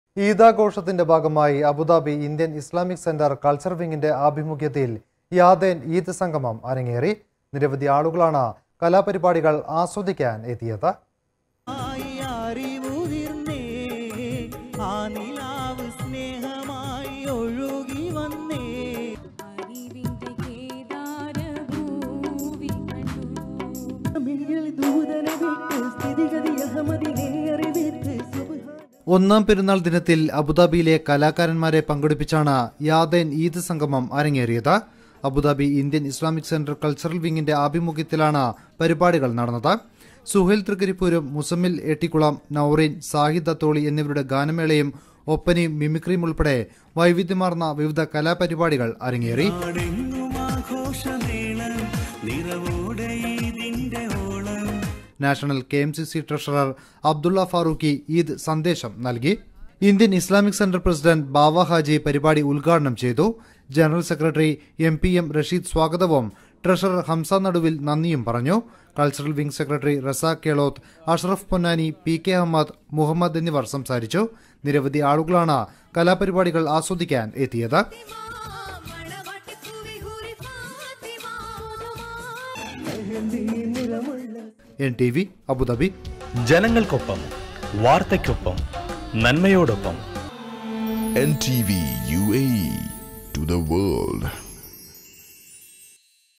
അബുദബി ഇന്ത്യന്‍ ഇസ്ലാമിക് സെന്റര്‍ കള്‍ച്ചര്‍ വിംഗിന്റെ ആഭിമുഖ്യത്തില്‍ യാദേന്‍ ഈദ് സംഗമം അരങ്ങേറി
ഈദാഘോഷത്തിന്റെ ഭാഗമായി അബുദബി ഇന്ത്യന്‍ ഇസ്ലാമിക് സെന്റര്‍ കള്‍ച്ചര്‍ വിംഗിന്റെ ആഭിമുഖ്യത്തില്‍ യാദേന്‍ ഈദ് സംഗമം അരങ്ങേറി.
നിരവധി ആളുകളാണ് കലാപരിപാടികള്‍ ആസ്വദിക്കാന്‍ എത്തിയത്.